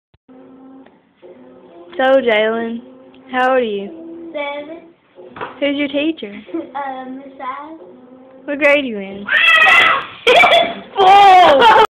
GHOST DRAG
EVERY TIME MY NIECE,BELLA, LAYS IN THIS SPOT A GHOST DRAGS HER ALL THE WAY DOWN THE HALLWAY AND STOPS IN FRONT OF THE BATHROOM DOOR.